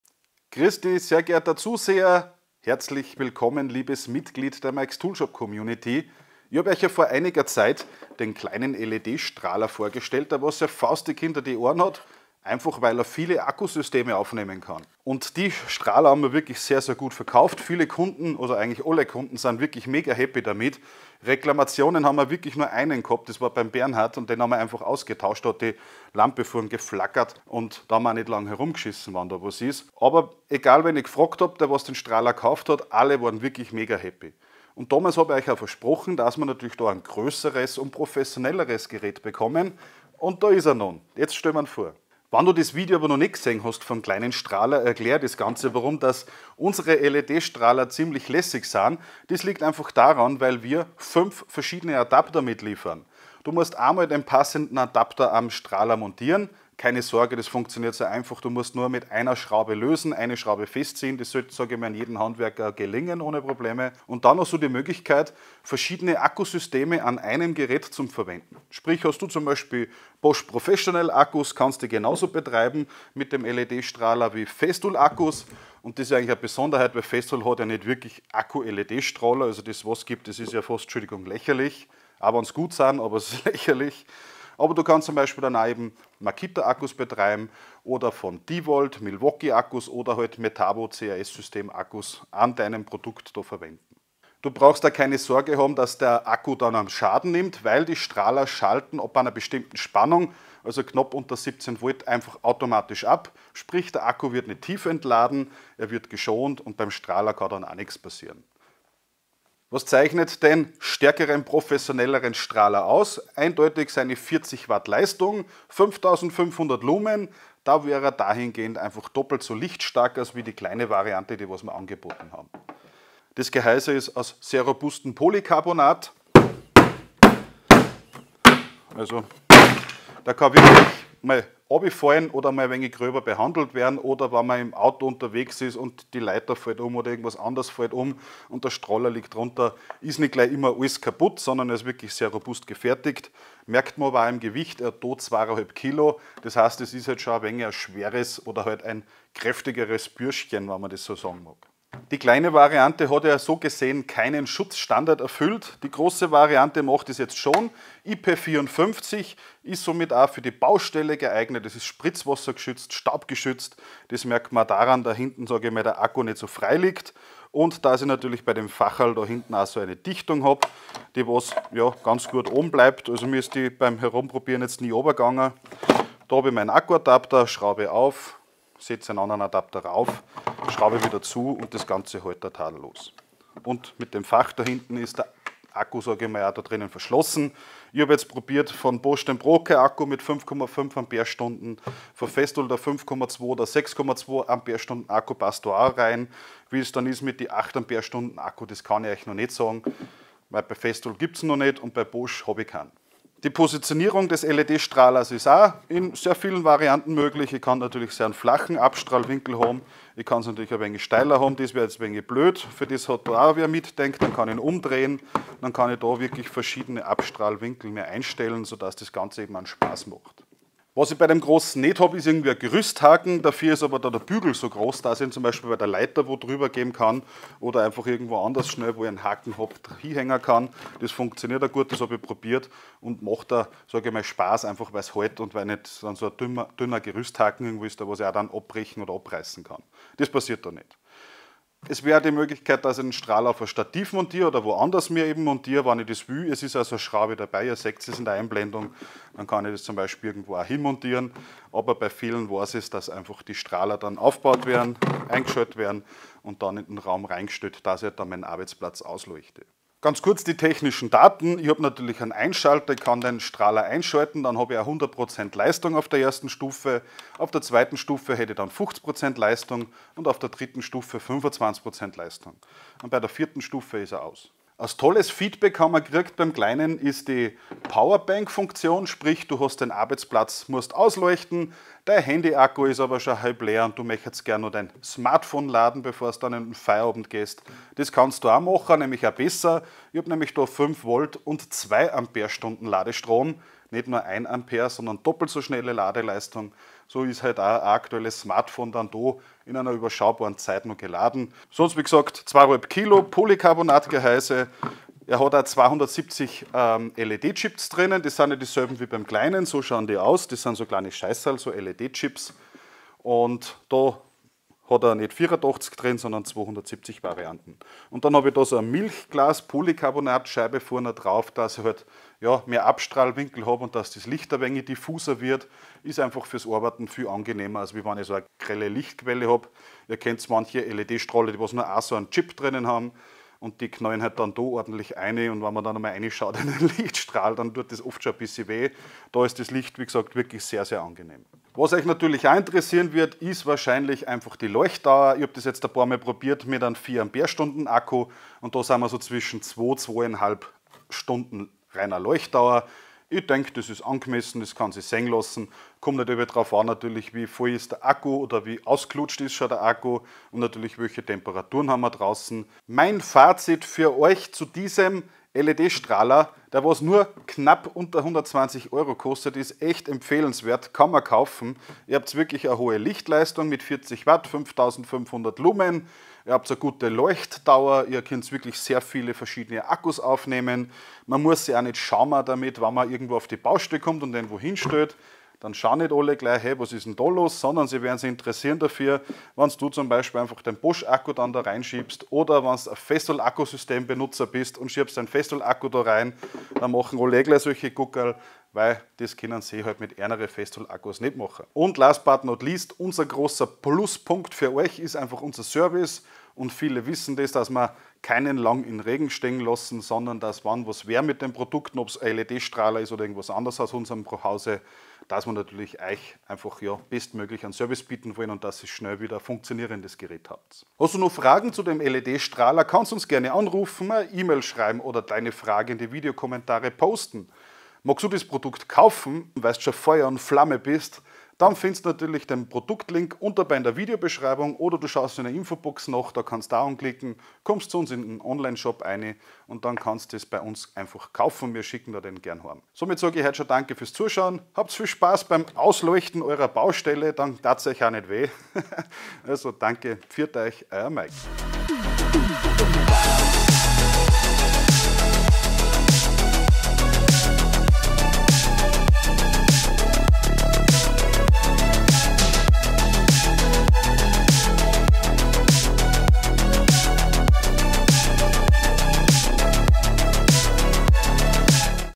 PROFI LED Arbeitsstrahler für Festool, Bosch, Makita, Metabo, CAS, Milwaukee... - Mikes Toolshop
Jetzt ist der große Profi LED-Strahler endlich da! Er ist guuut!
Mit deinem Einkauf unterstützt du uns aktiv.

Impressum:
Mikes Toolshop GmbH
Gewerbegebiet 6
AT-5121 Ostermiething